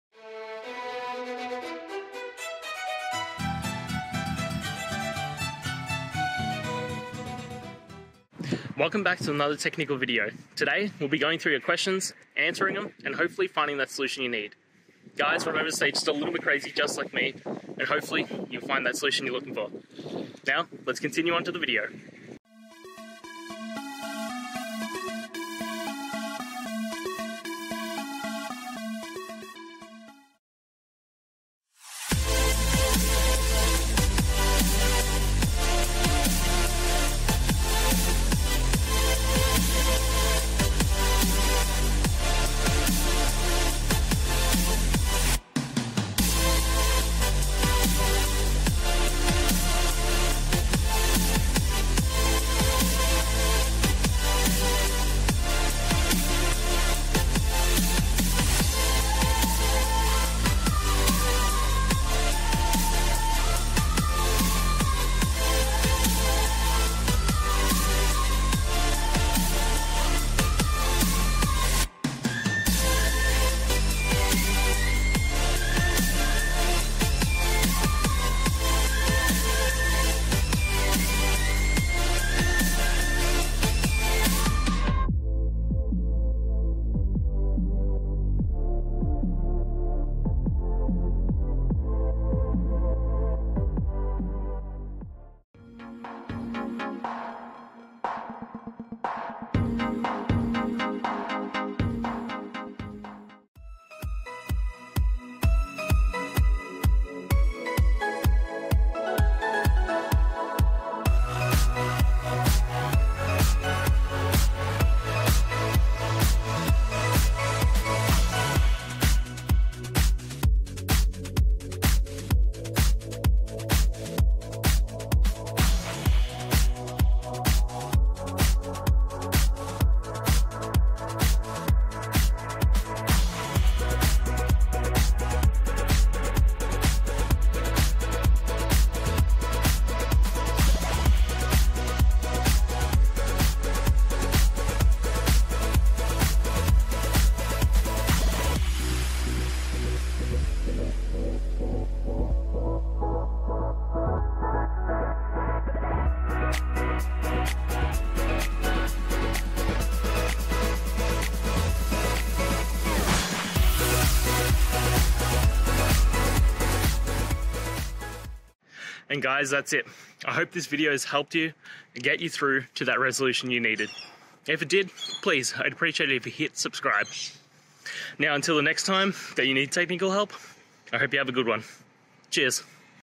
Understanding Perl 6 Map: Why It Returns Value vs List in Different Cases
In this video, we delve into the intricacies of Perl 6's map function, exploring its behavior in returning values versus lists in various scenarios. Whether you're a seasoned programmer or new to Perl 6, understanding these nuances is crucial for effective coding. Join us as we break down the mechanics behind map, providing clear examples and insights to enhance your programming skills.

Today's Topic: Understanding Perl 6 Map: Why It Returns Value vs List in Different Cases

Related to